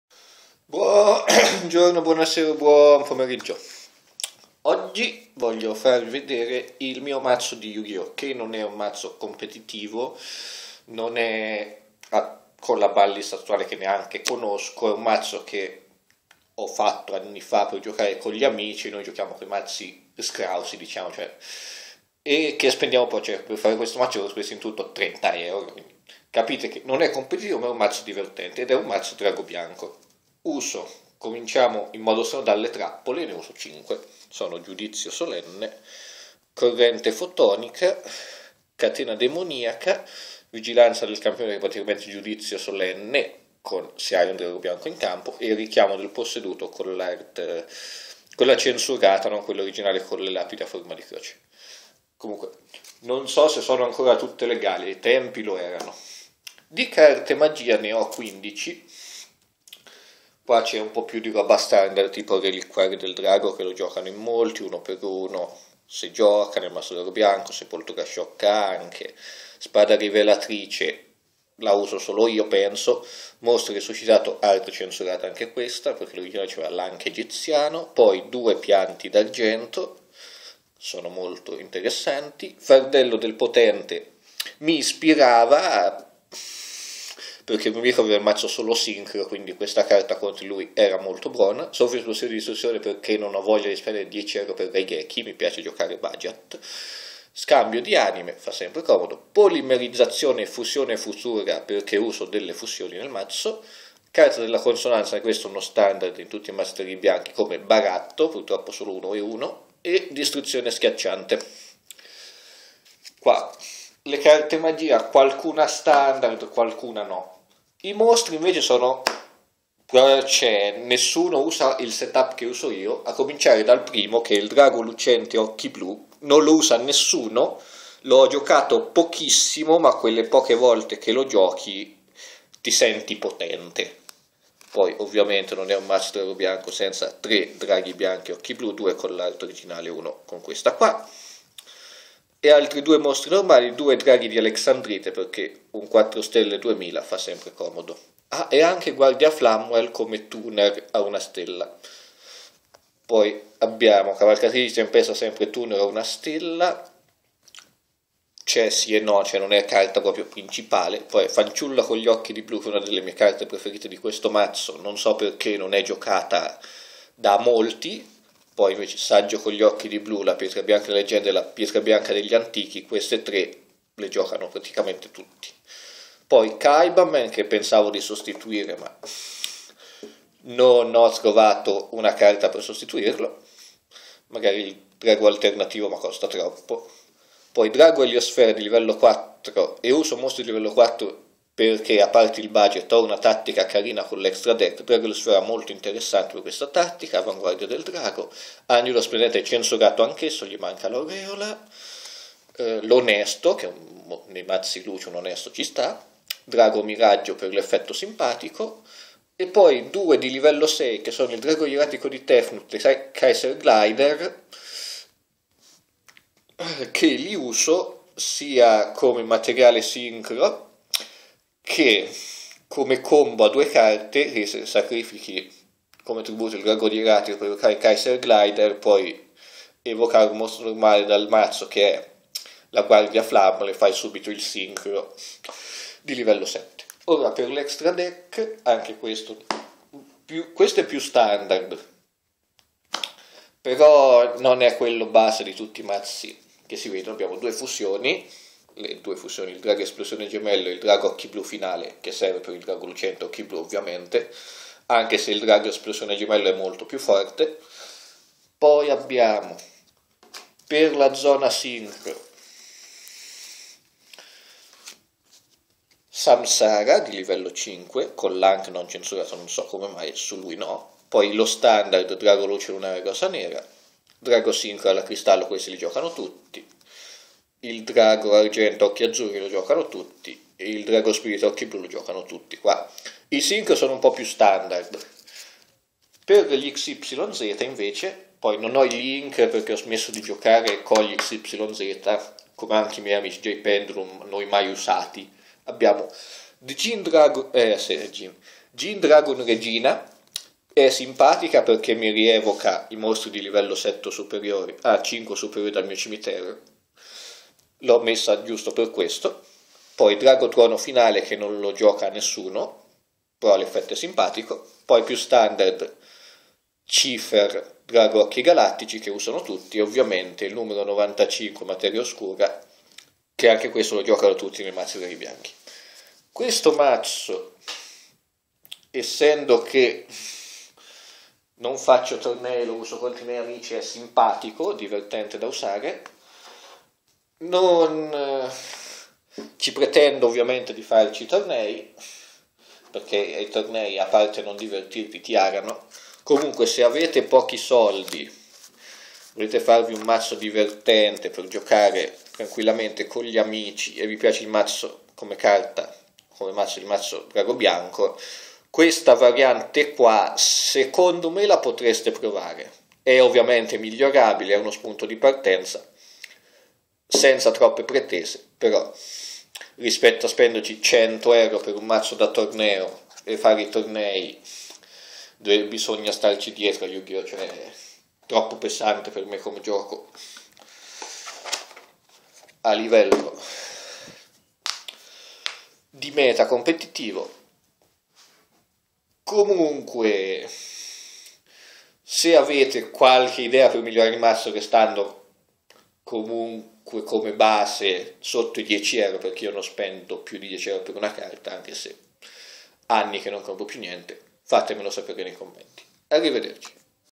Mazzo drago bianco Yu-gi-oh
Il mio mazzo di Yu-gi-oh fatto anni fa, molto budget e non competitivo ma divertente per partite fra amici senza troppe pretese